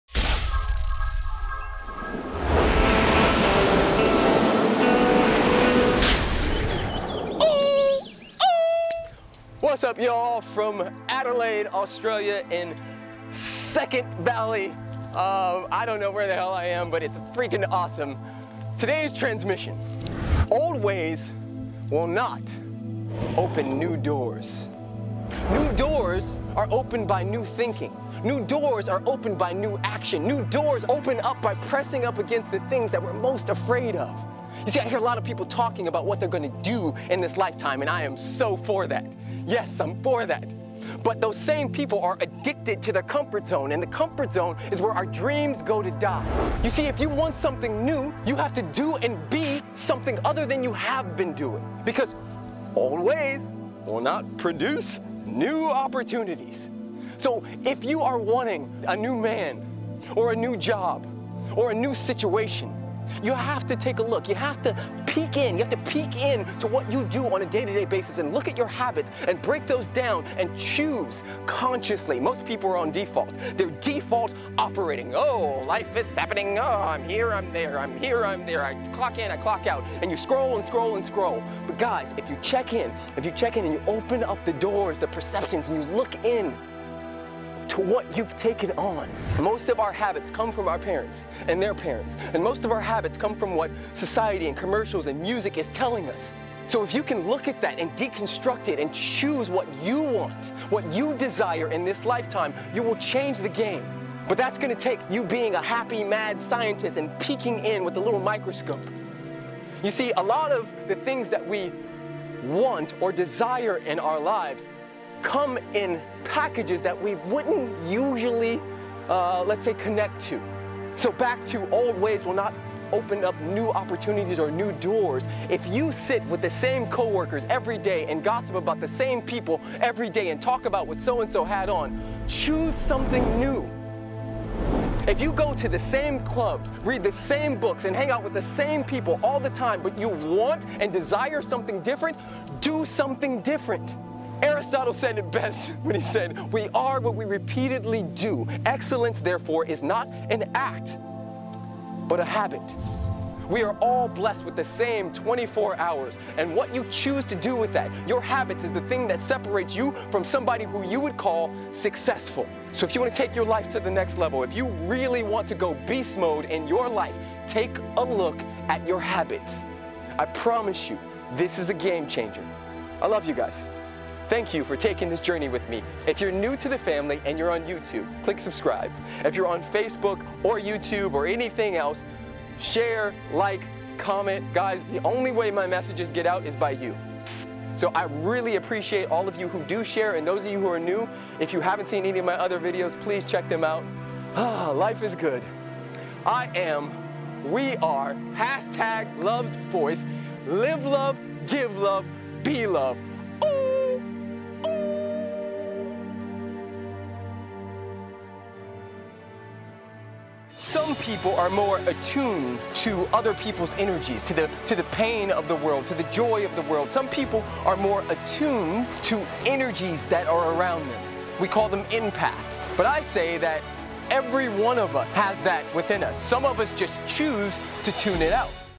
Next Level Thinking-- the ONE thing Successful people get
Old ways will not open NEW doors. This video is about creating new habits